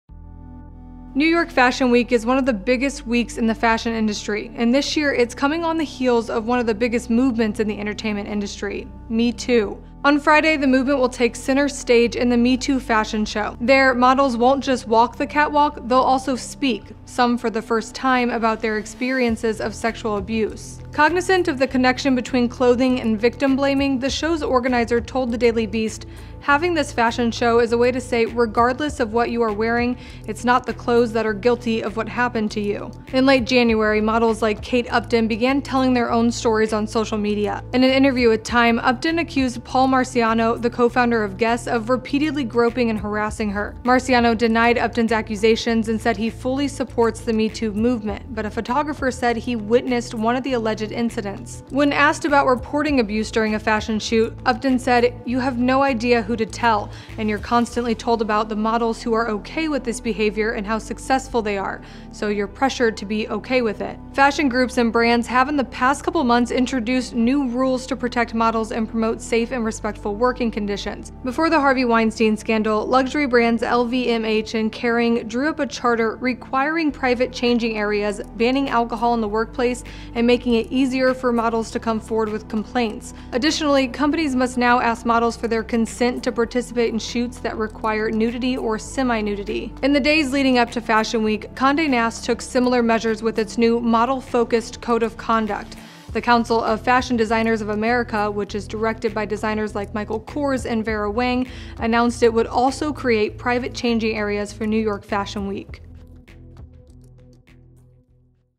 MeToo is a big part of New York Fashion Week
Models are coming forward with their stories of sexual abuse, and major fashion groups are working to create safe, respectful working environments.